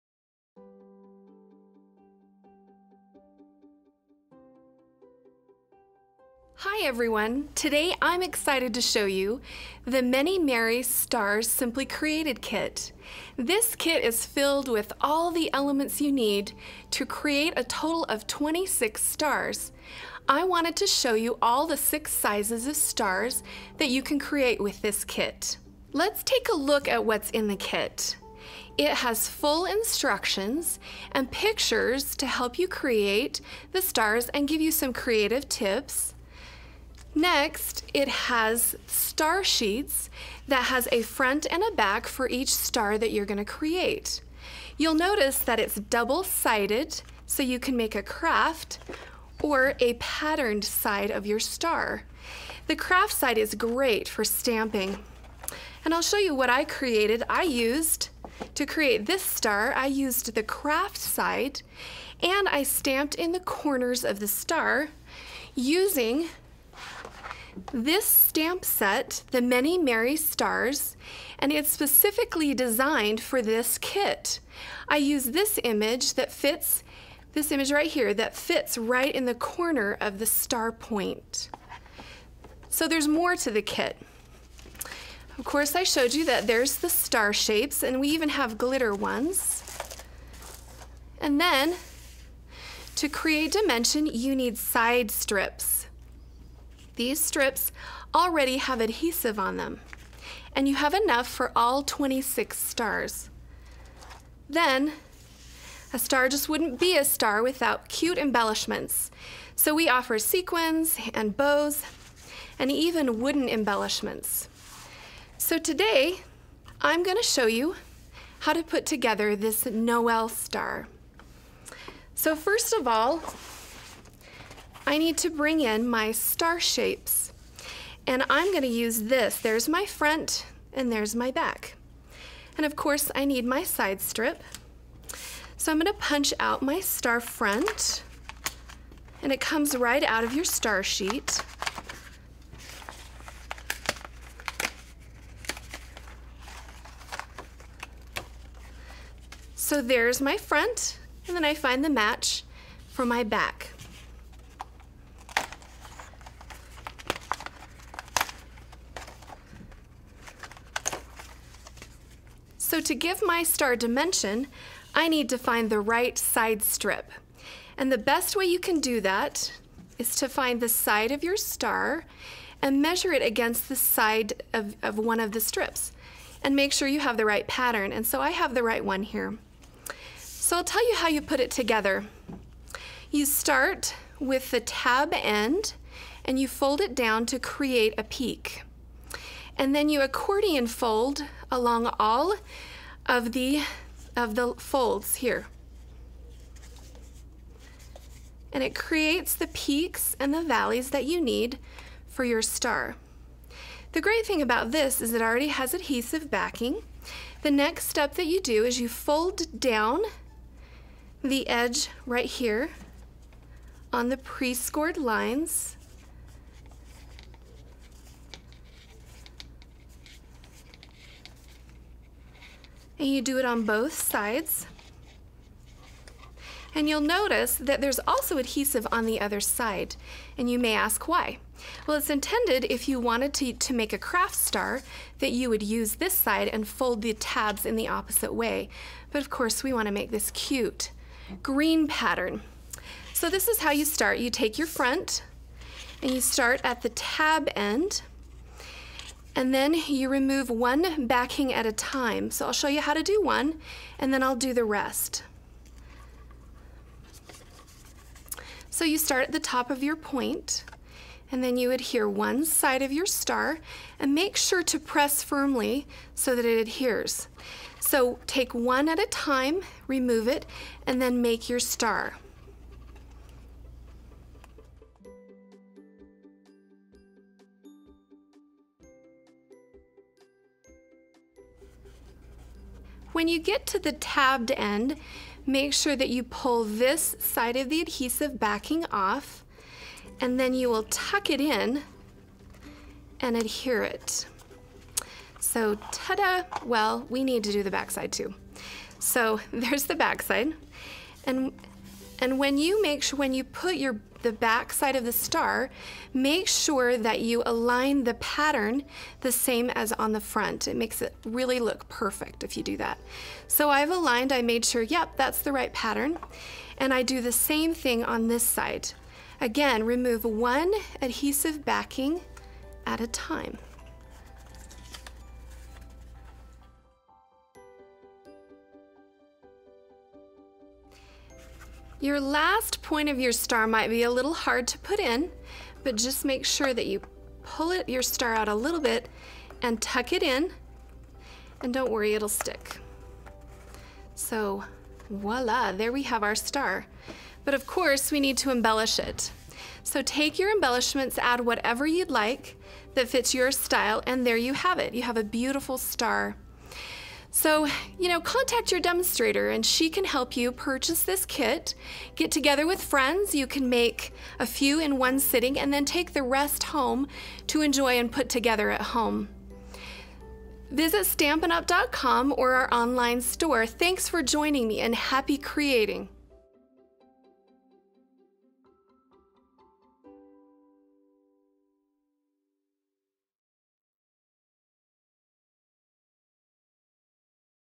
Many Merry Stars Simply Created® Kit
Count your lucky stars! Create 26 stars perfect for holiday uses from tabletop to tree. The dazzling Many Merry Stars Simply Created ® Kit includes everything you need, including exclusive accessories like wood cutouts and bows.